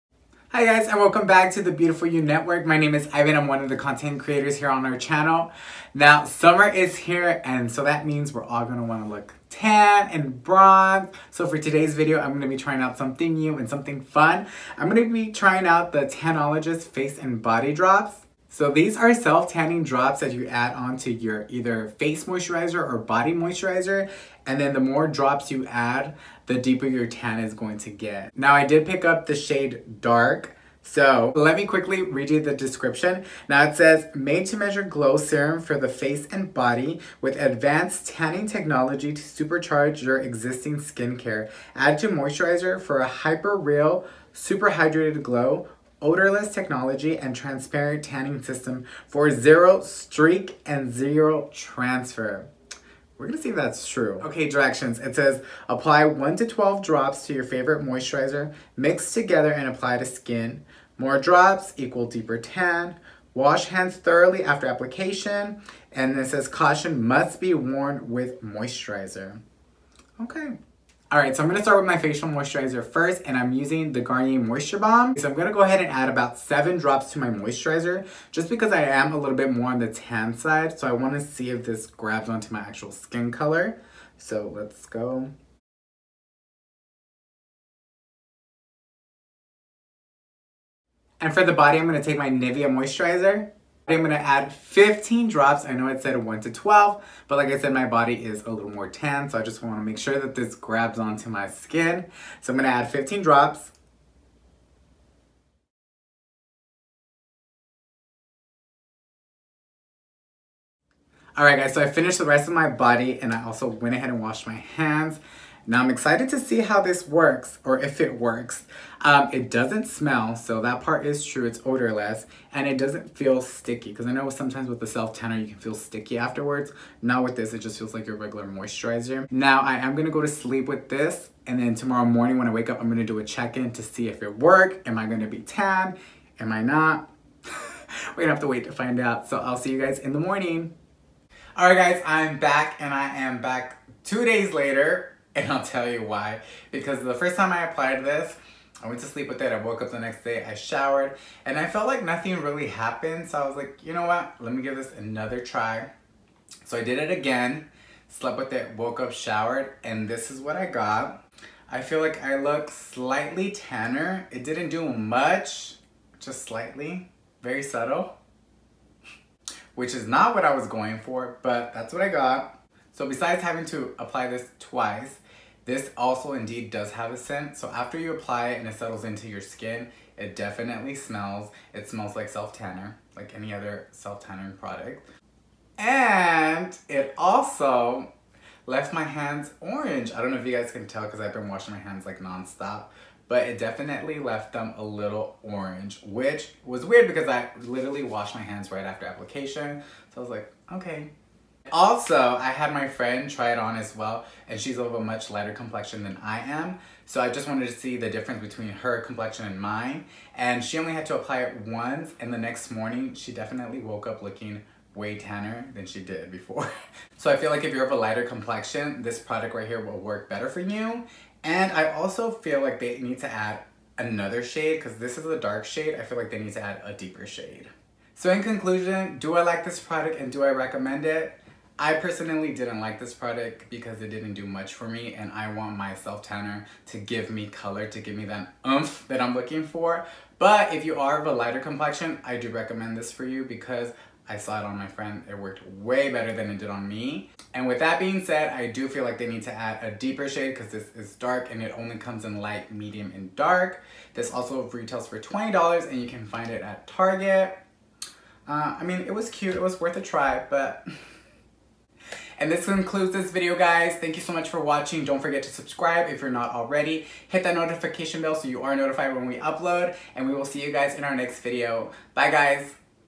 NEW DRUGSTORE TANOLOGIST TANNING DROPS | Do they really work?!?
Hey ya'll!
Who wants to be tan for summer with out the burn!? We all know the sun ages us and we do not want that! No sir!  In today's video I try out the new tanning drops from Tanologist! Keep watching to see my honest thoughts!